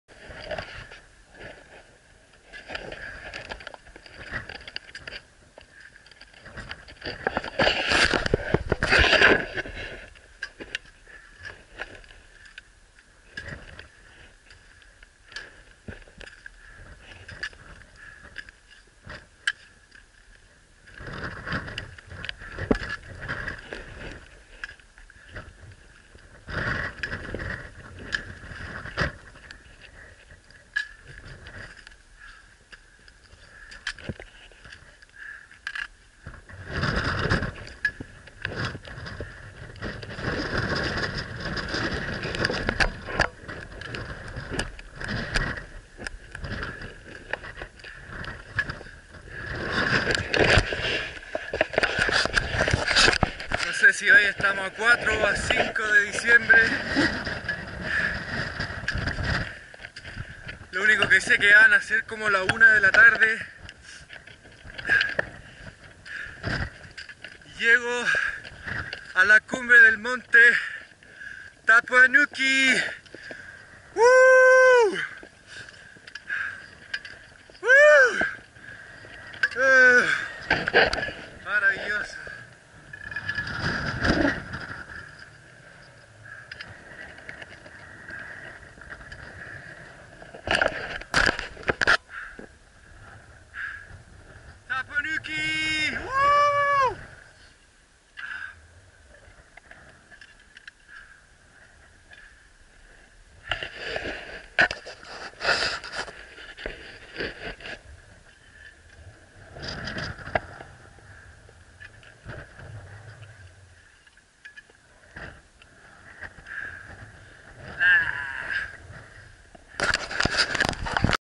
New Zealand. Tapuae O Uenuku Cumbre!
Tapuae O Uenuku is the highest peak in the Inland Kaikouras, and also the highest peak in NZ outside of the Mt Cook chain. It is a fine alpine mountain, offering stunning views, yet technically very accessible.

The main route is non-technical, but does ascend steep snow slopes and a ridge, so experience of using crampons and an ice axe is necessary.

It is generally climbed in 3 days accessed via the Hodder River.